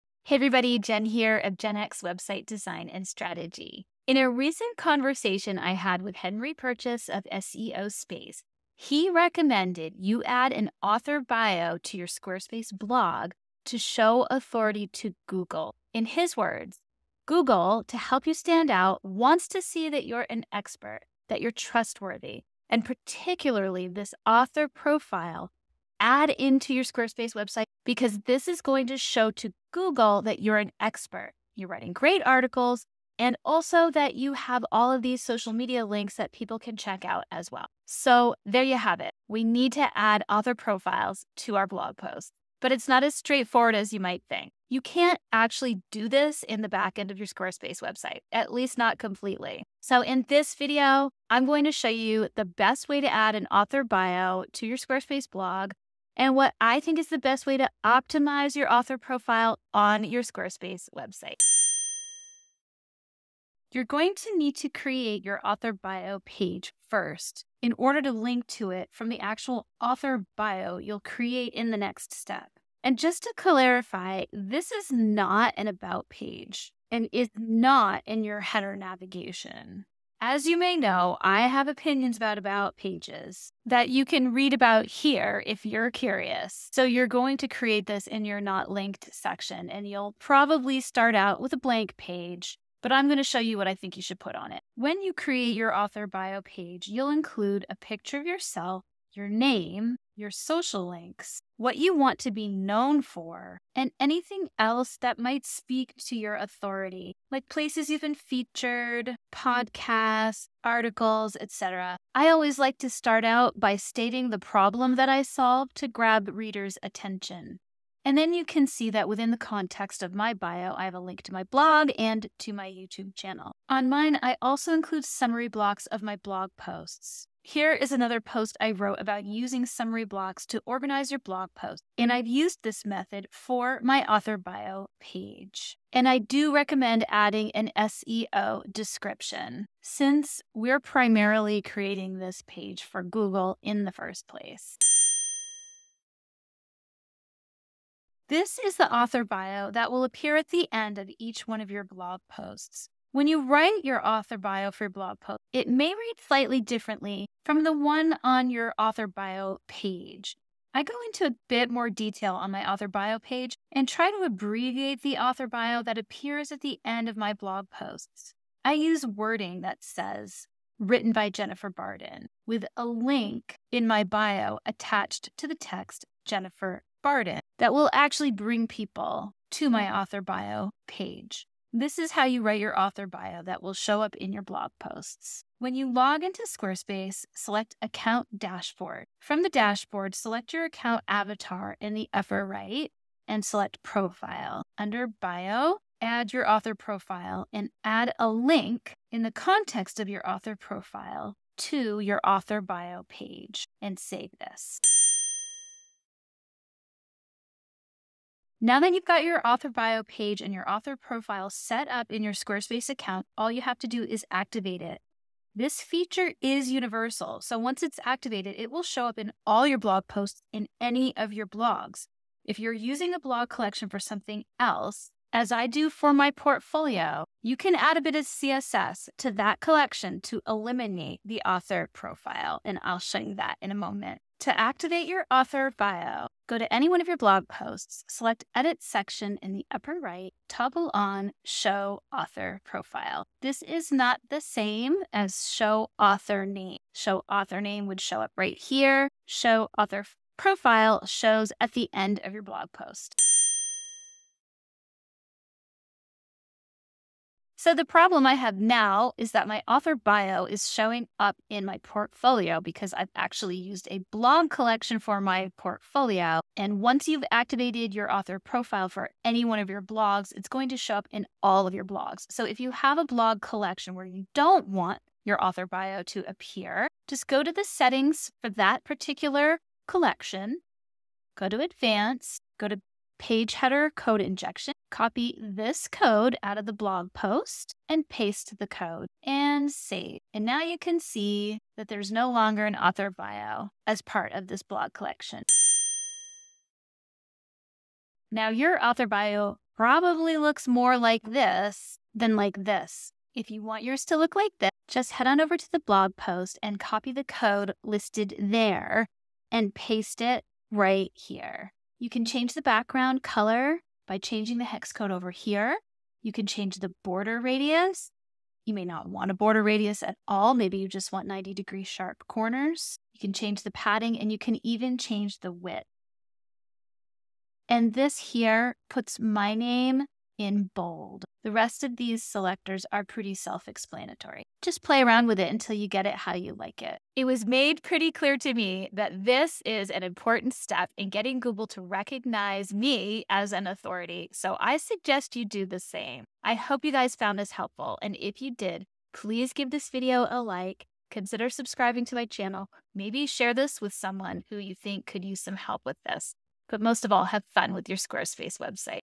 Add an Author Bio to Your Squarespace Blog to Show Authority
This step-by-step tutorial shows you how to add an author bio to your Squarespace blog to show authority to Google so that Google will recognize you as an expert at what you do.

1:01 Create an author bio page
2:25 Write your author bio in your Squarespace dashboard
3:25 Add the author bio to your Squarespace blog
4:13 What to do if you don't want your author bio in all your blog collections
4:55 Customize your author bio with custom CSS

👉 How to Establish Authority on Your Website With Your Potential Customers